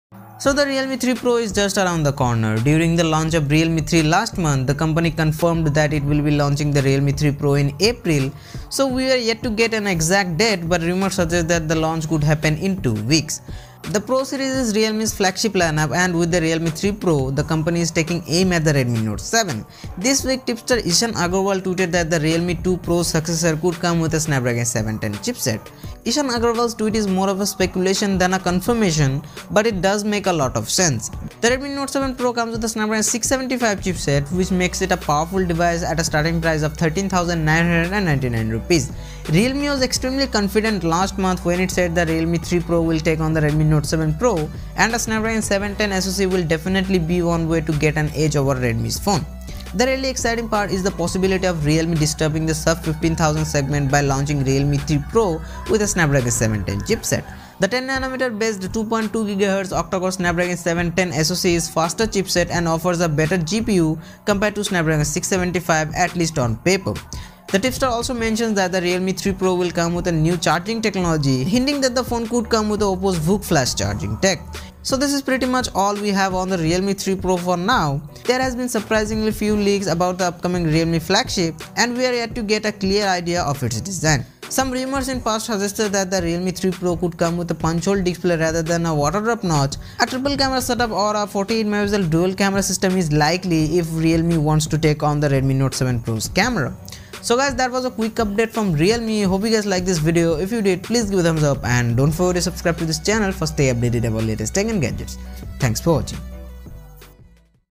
Realme 3 Pro - Coming With Snapdragon 710 ?
The Realme 3 Pro is just around the corner. During the launch of the Realme 3 last month, the company confirmed that it will be launching the Realme 3 Pro in April. We are yet to get an exact date, but rumours suggest the launch could happen in two weeks. The Pro series is Realme's flagship line, and with the Realme 3 Pro, the company is taking aim at the Redmi Note 7 Pro. This week, tipster Ishan Agarwal tweeted that the Realme 2 Pro successor could come with a Snapdragon 710 chipset.